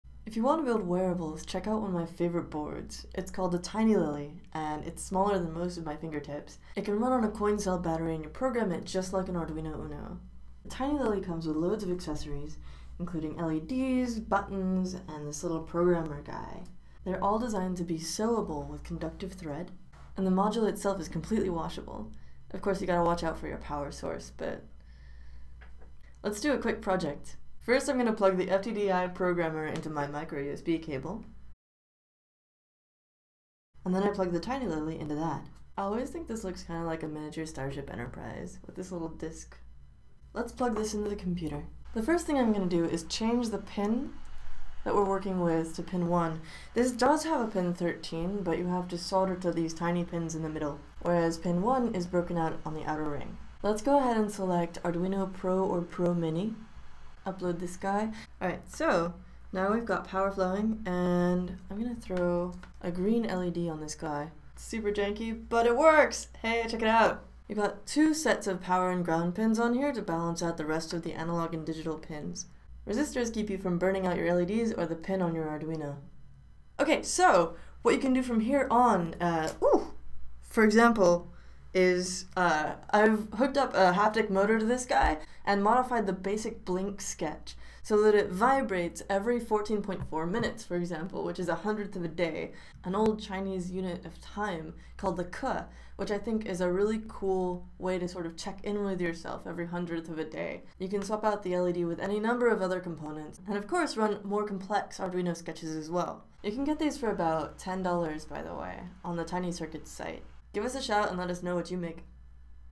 Arduino Basics: TinyLily for Wearable Tech
Sewable, washable, and extremely small: start building your own wearable devices with the TinyLily Arduino!